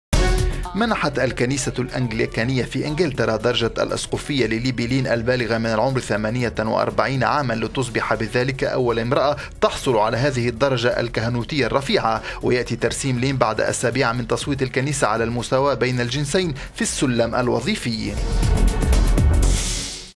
ترسيم اول امراة أسقفا في الكنيسة الانغليكانية ببريطانيا
منحت الكنيسة الانغليكانية في انكلترا درجة الاسقفية لليبي لين البالغة من العمر 48 عاما لتصبح بذلك أول إمراة تحصل على هذه الدرجة الكهنوتية الرفيعة. ويأتي ترسيم لين بعد اسابيع من تصويت الكنيسة الانغليكانية على المساواة بين الجنسين في السلم الوظيفي.…